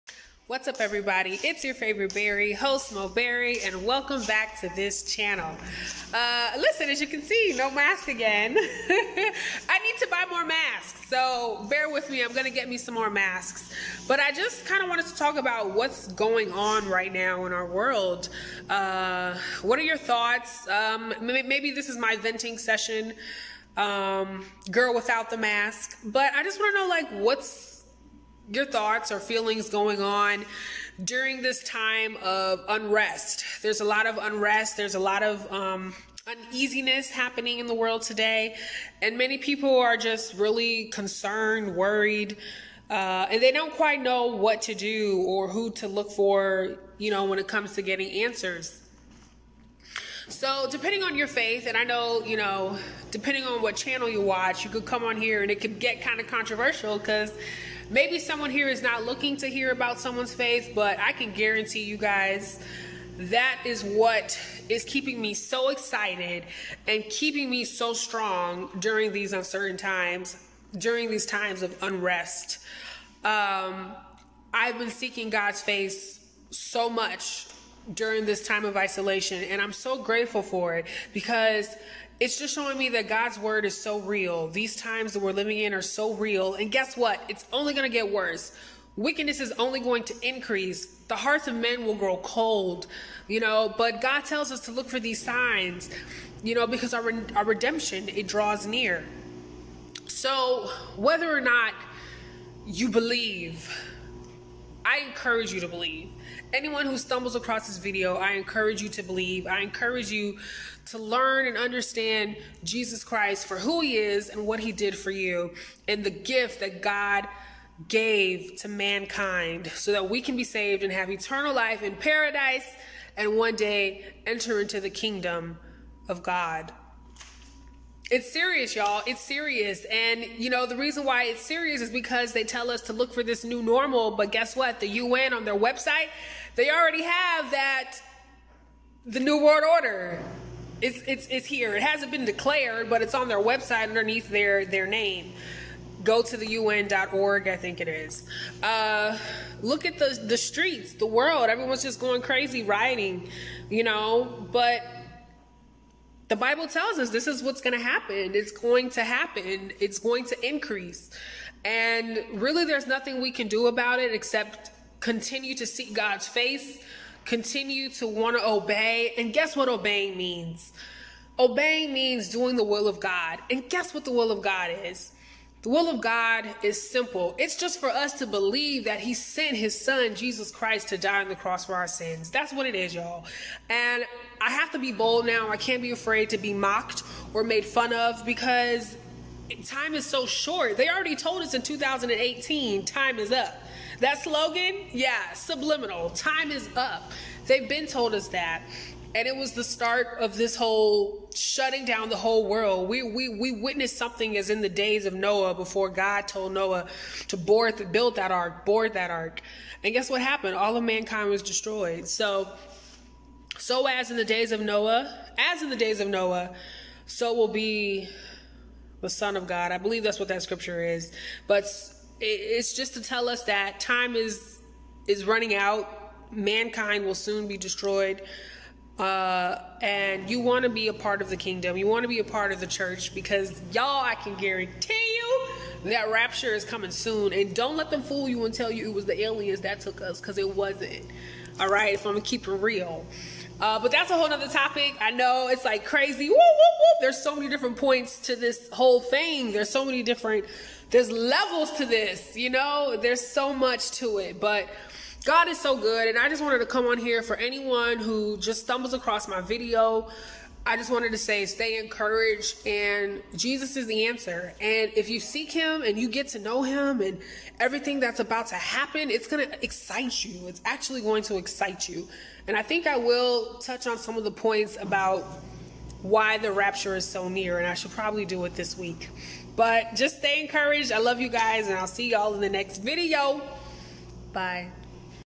Do Not Be Afraid To Be Mocked | Host Mo Berry
Time is up they say. God's will is so easy, He called us all to believe in the precious gift of Jesus, who died & was persecuted just so you may have everlasting life in Paradise.